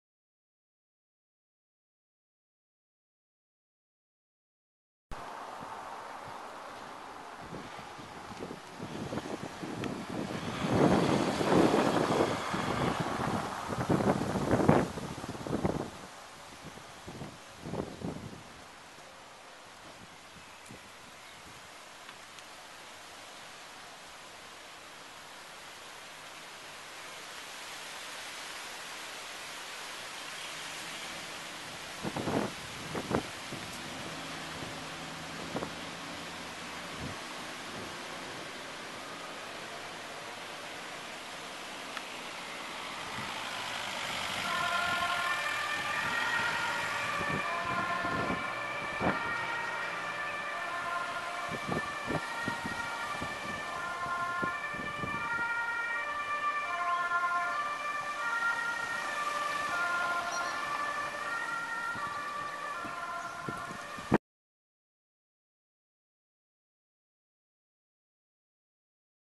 23-05-2011 A1 25-115 Richting ziekenhuis
Las op twitter dat er een Ambu in Ens was en ben even gaan kijken, weet verder niet wat er aan de hand was